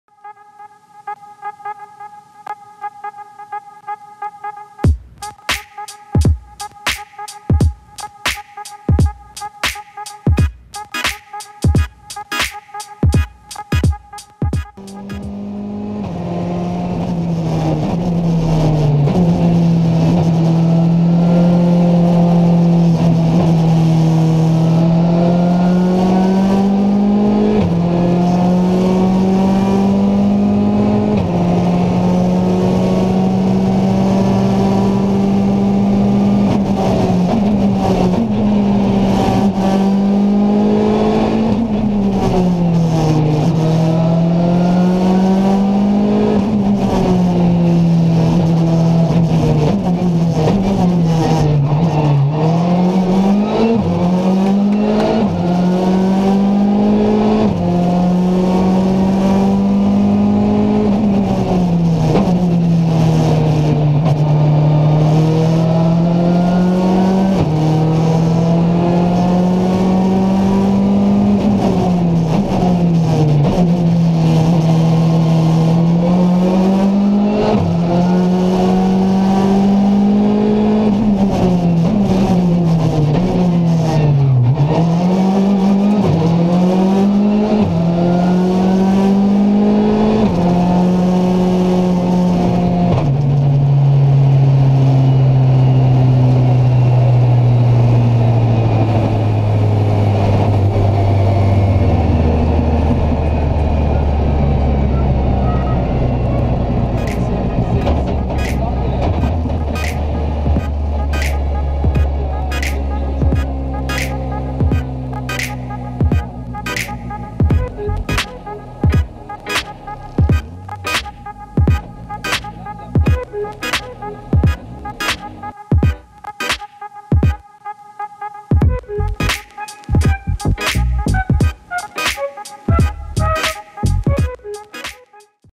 Essai sur piste de la Skoda Fabia R5
Direction le circuit d'Issoire pour tester la Skoda Fabia R5, forte de 265 ch pour seulement 1230 kg, et de quatre roues motrices !